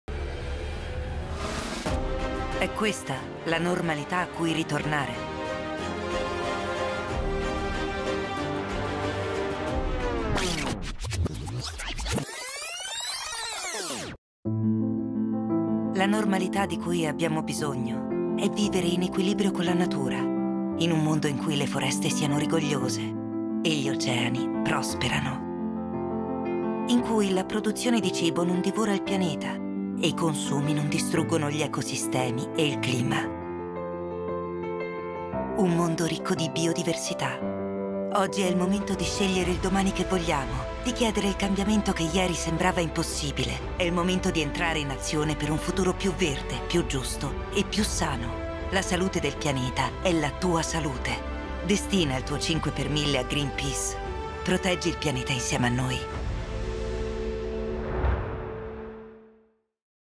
Proteggi il Pianeta insieme a noi - Dona il 5x1000 a Greenpeace
È questa la normalità a cui ritornare?
La normalità di cui abbiamo bisogno
È vivere in equilibrio con la natura
In un mondo in cui le Foreste siano rigogliose
E gli Oceani prosperano
In cui la produzione di cibo non divora il Pianeta
E i consumi non distruggono gli ecosistemi e il clima.
Un mondo ricco di biodiversità.
Oggi è il momento di scegliere il domani che vogliamo
Di chiedere il cambiamento che ieri sembrava impossibile.
È il momento di entrare in azione per un futuro più verde, più giusto e più sano.
La salute del pianeta è la tua salute.
Proteggi il Pianeta insieme a noi.
Destina il tuo 5 per 1000 a Greenpeace.